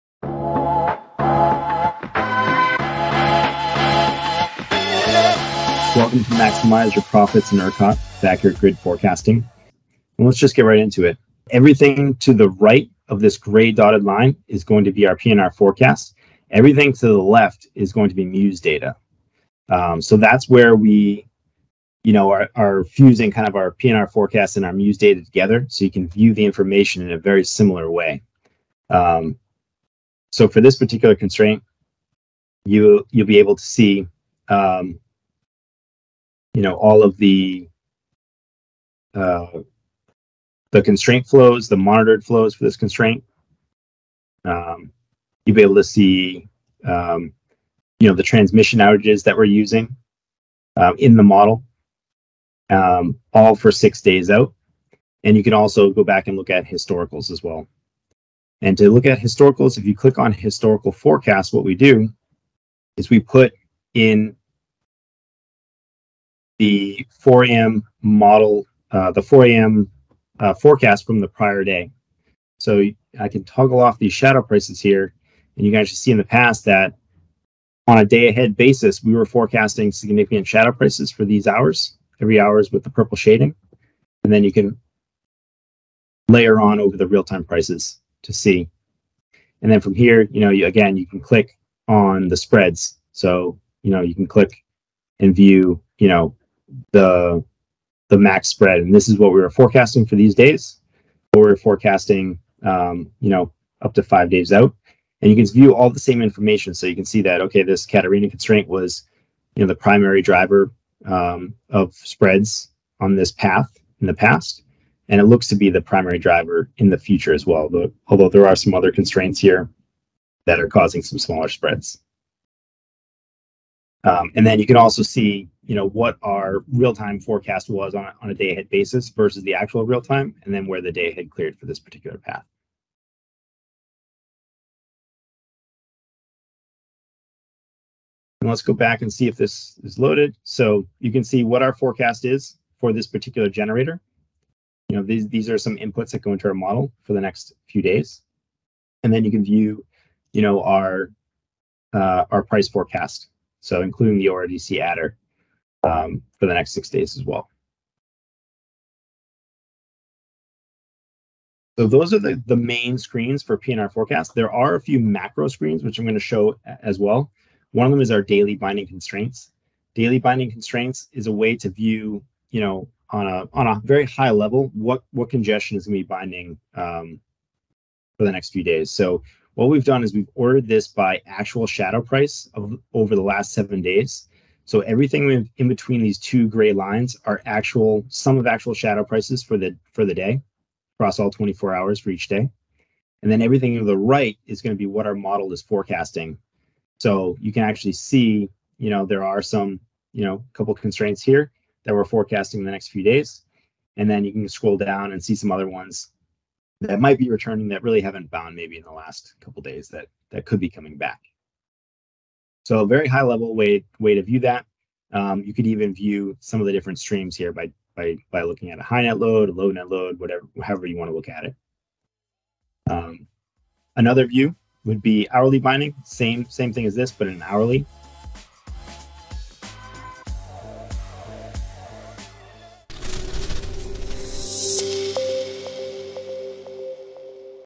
Enverus Power & Renewables Forecast in ERCOT
P&R Forecast provides high-quality forecast analytics for every element on the grid, from power generation to individual nodes. It utilizes automated production cost modeling to build a complete, simulated market view with price forecasts, generation dispatch, transmission information, and congestion decomposition, helping traders and asset managers build understanding and confidence in the market.